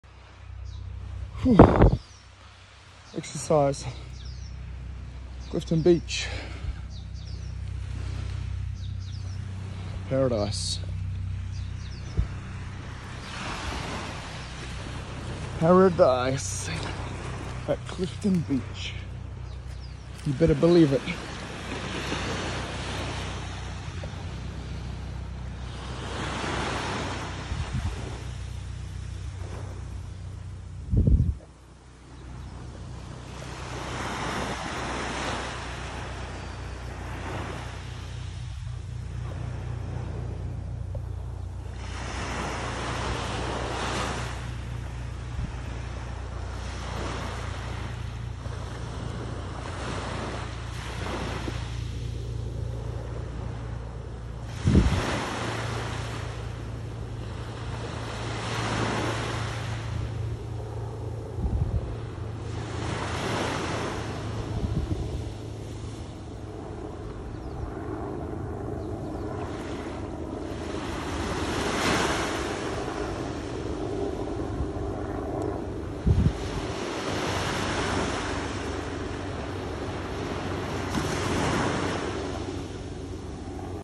Early morning exercise, Glorious Clifton Beach Qld Aus Irukandji jellyfish rescue Chopper !!!
Early morning exercise at the glorious Clifton Beach, Cairns Qld Aus. Getting some DAWN sunrise shots , rescue helicopter flys by to save someone dying from IRUKANDJI JELLYFISH STING !